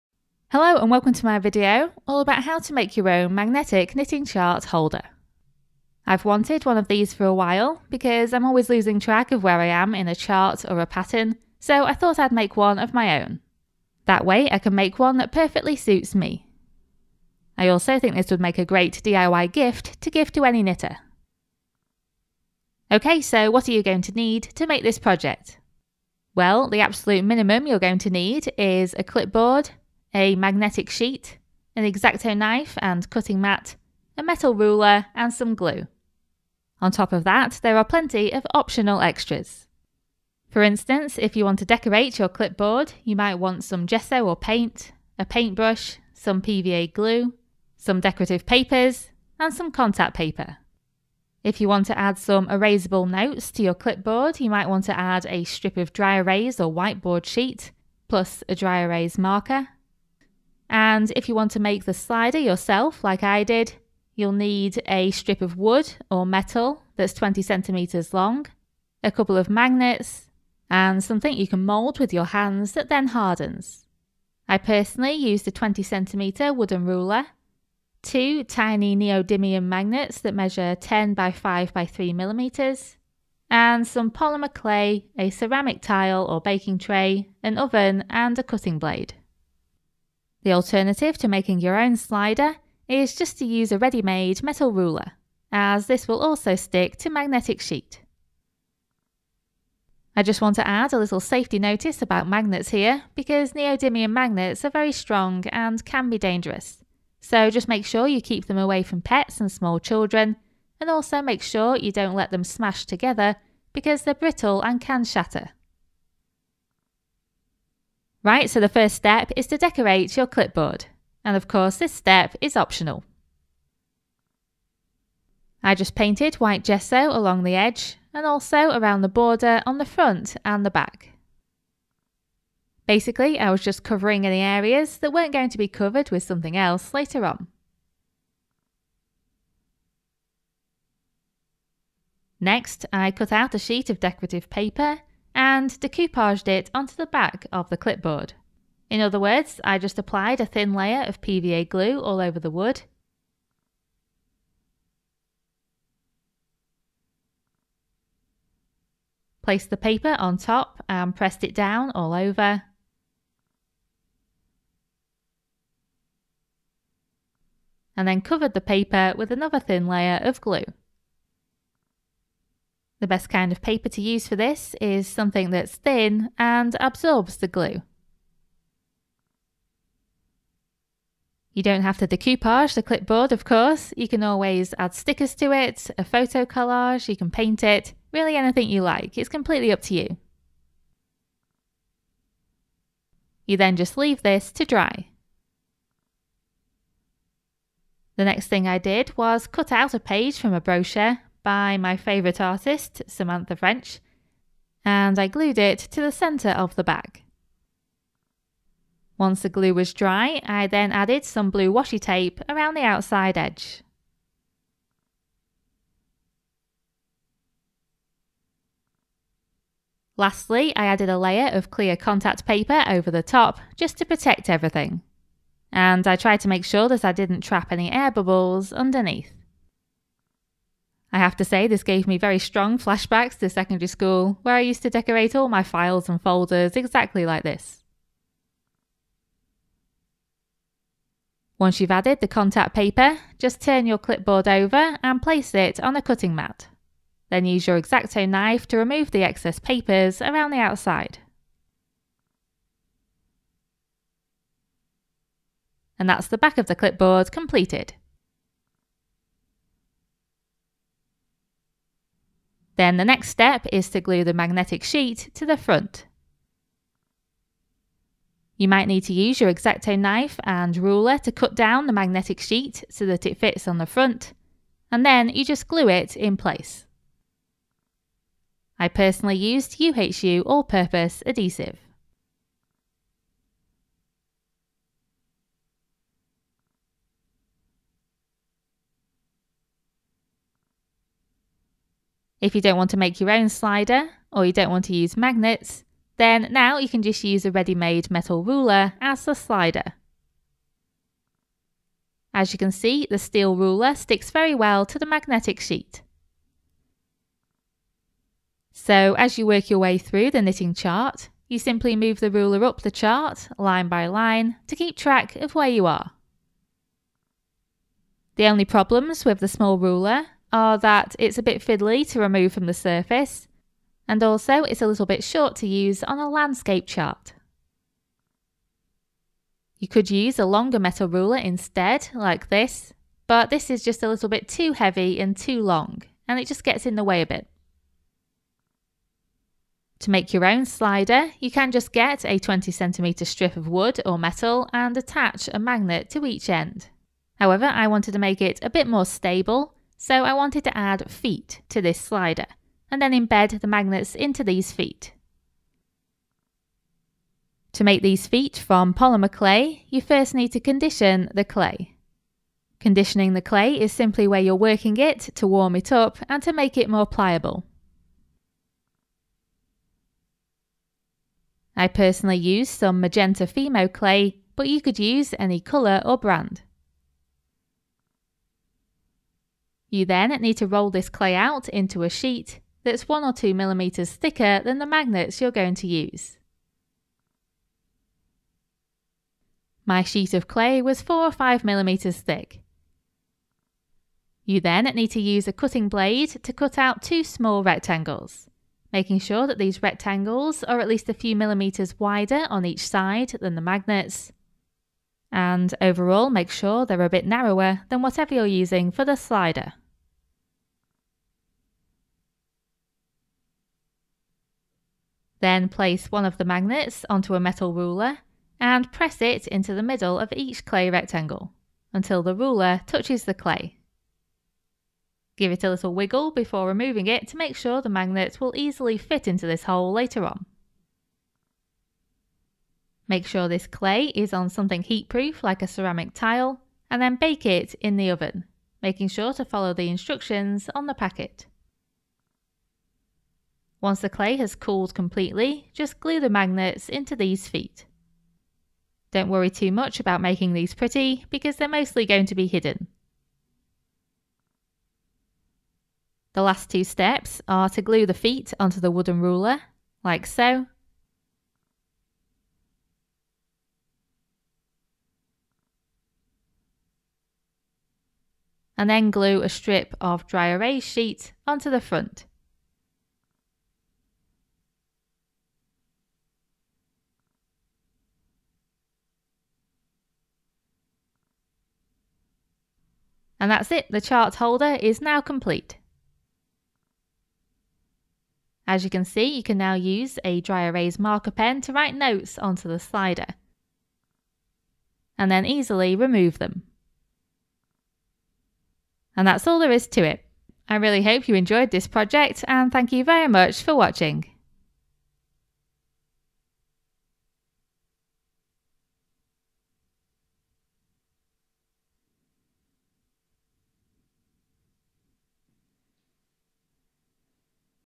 DIY MAGNETIC KNITTING PATTERN HOLDER | How to Make a Handy A4 Chart Keeper | Gift for Knitters
This quick and easy project uses a cheap clipboard as the base, and I'm sure the end result would be a useful addition to any knitter's inventory :)

Magnetic sheet and Neodymium magnets keep the 'line keeper' in place on the front of your knitting pattern or chart, so you can keep track of which line you are on...and I also add a strip of whiteboard so you can write erasable notes to yourself too.

I hope you like this DIY!

00:00 What You Will Need
01:56 Decorating the Clipboard
04:25 Adding the Magnetic Parts

Any support is hugely appreciated and helps me to cover websites fees, material costs and other expenses related to creating videos. Thanks very much!